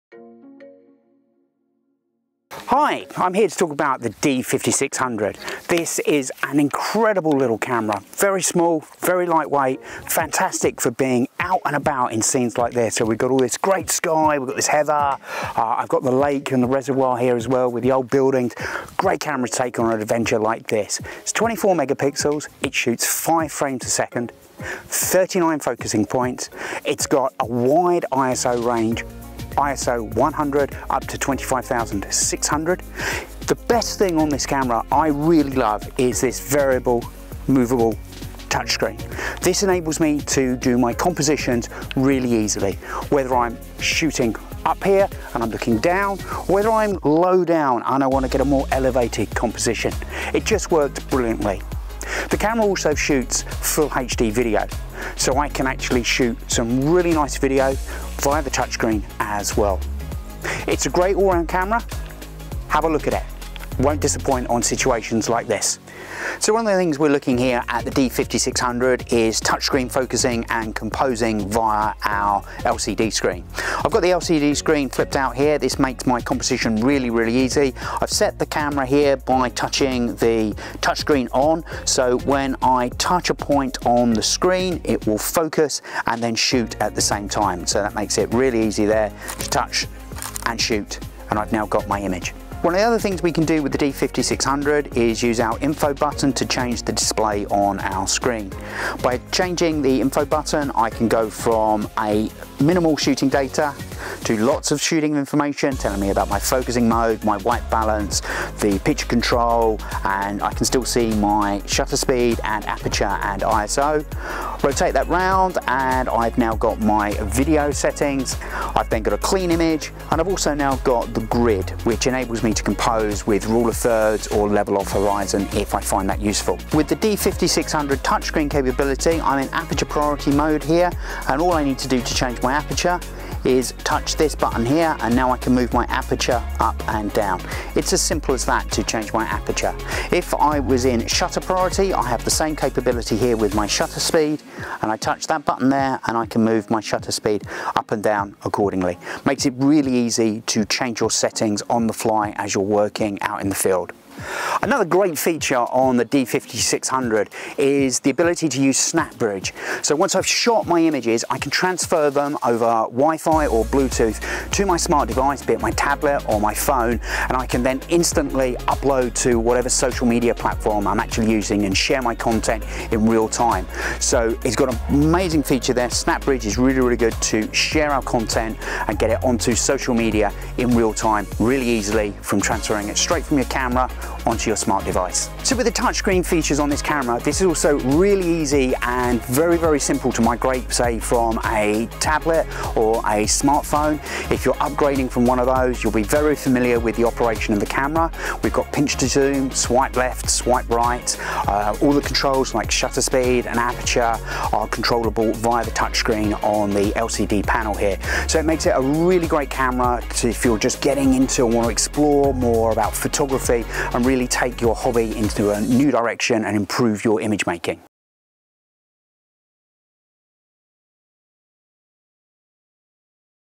Nikon School: Using the Nikon D5600
Neil Freeman, training manager at Nikon School UK, takes us through the key features of this camera body and where it’s best applied.

For more information on the Nikon School, to see what courses they offer, and how they can help you, visit their